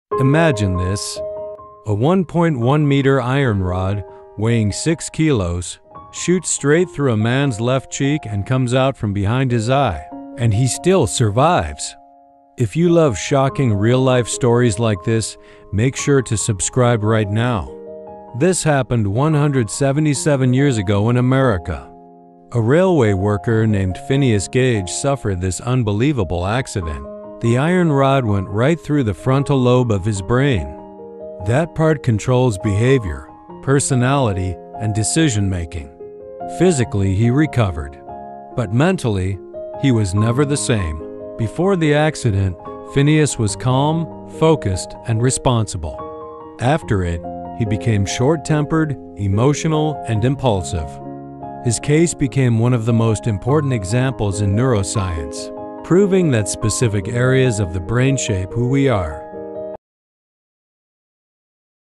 When a Metal Rod Changed a Man’s Personality Forever!”
177 years ago, a railway worker named Phineas Gage survived an accident that should’ve been impossible — an iron rod passed through his skull and brain, yet he lived! Music by capcut.
But his mind and personality completely changed.
Discover how one man’s tragedy helped science understand how the brain controls our behavior.

phineas gage story, brain injury miracle, neuroscience facts, real life medical case, psychology short, brain control personality, human brain experiment